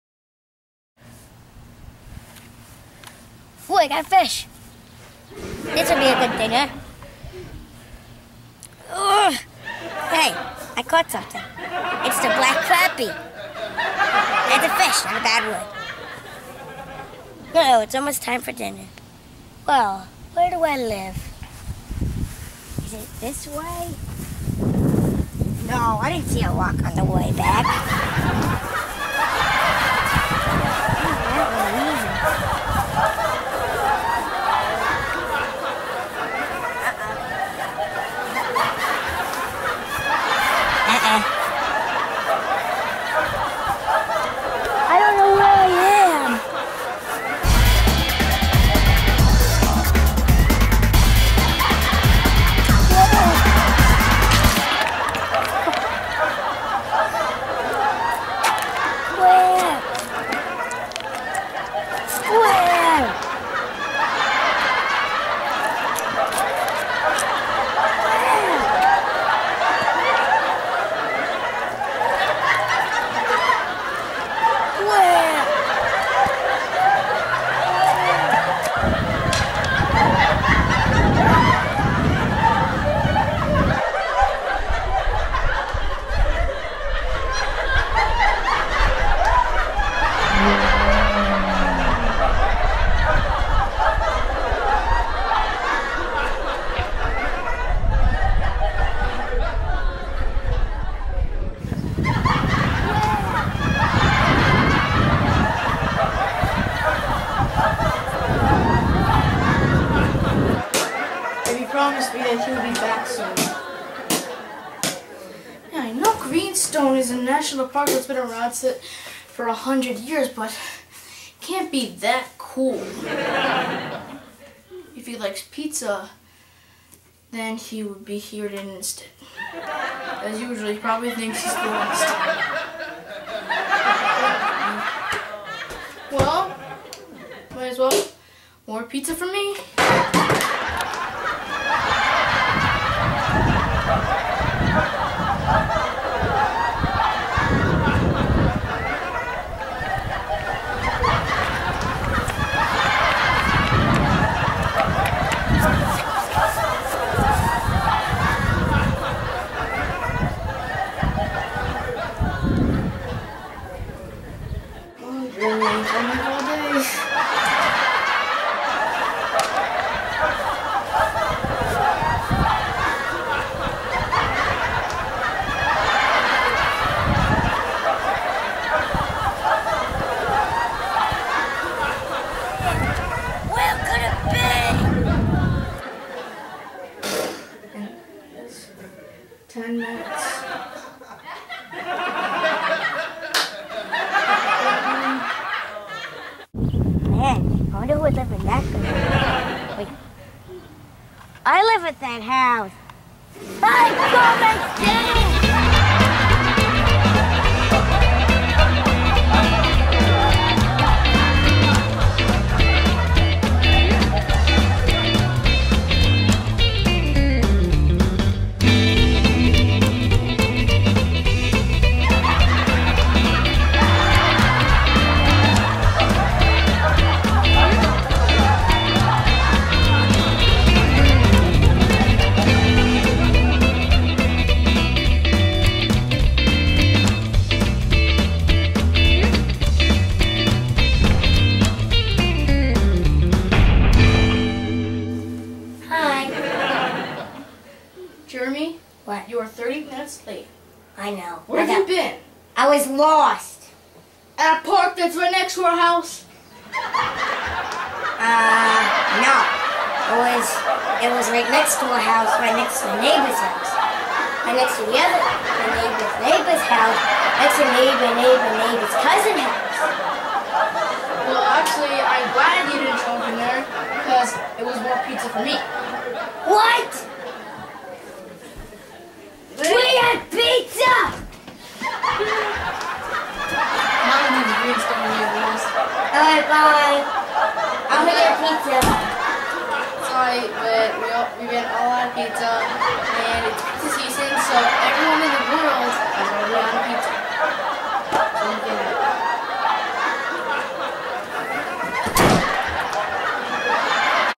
Lost in Transportation
It's almost dinner time, but Jeremy told Dan he would go fishing at Greenstone National Park and meet him there for dinner. But as usual, he's lost!